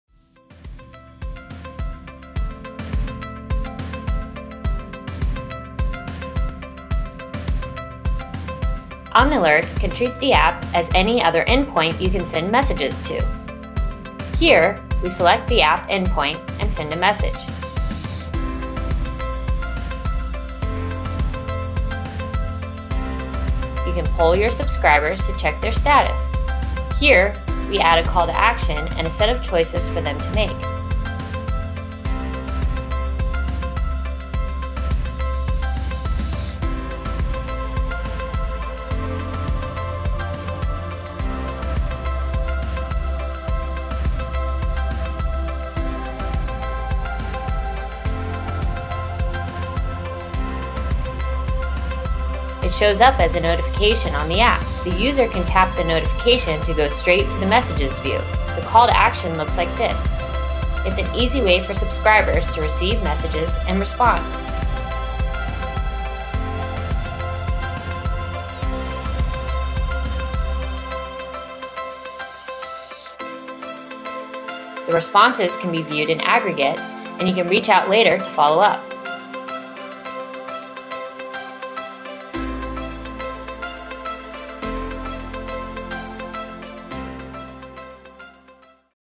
Omnilert App Demo - Sending Messages
Send a message with a call to action question and view the answers in aggregate.